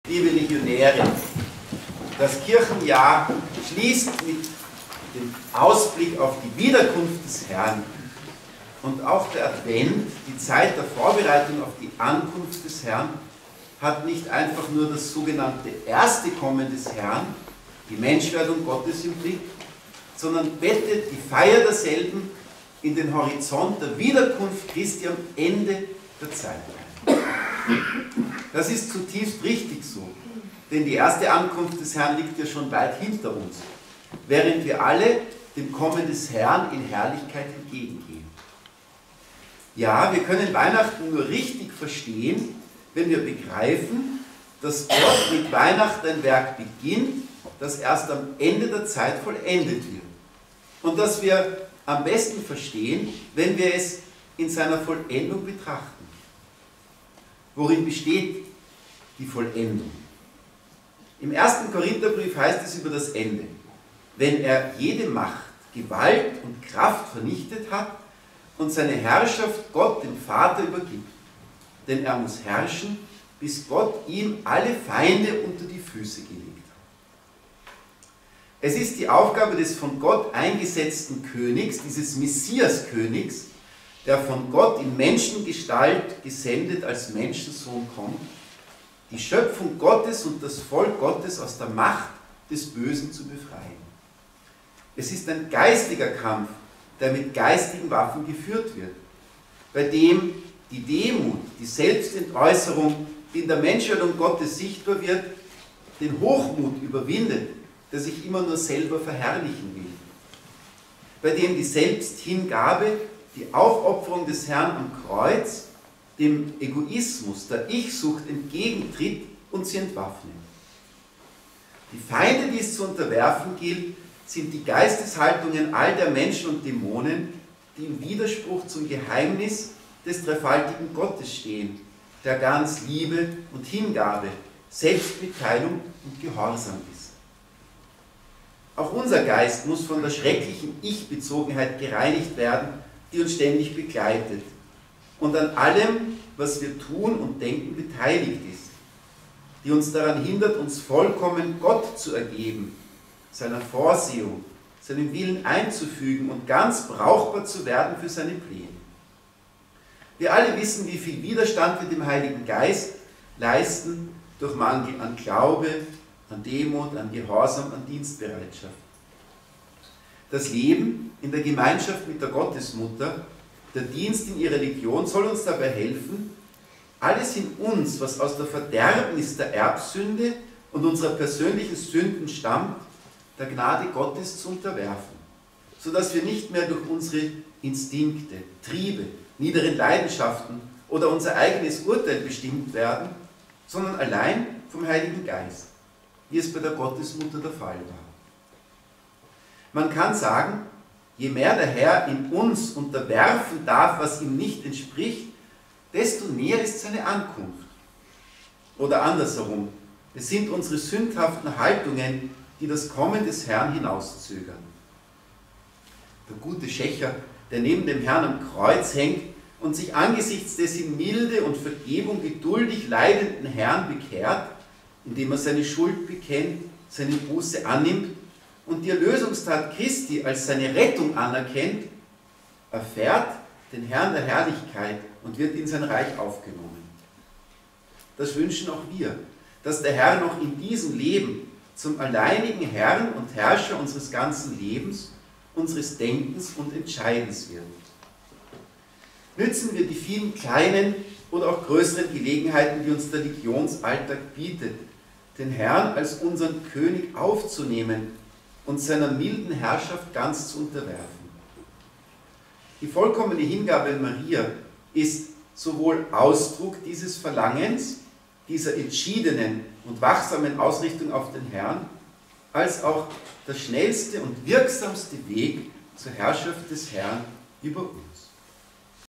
Allocutio beim Senatustreffen am 19. November 2022
Allocutio beim Senatustreffen am 19. November 2022 gehalten vom Geistlichen Leiter des Senatus, P. Florian Calice CO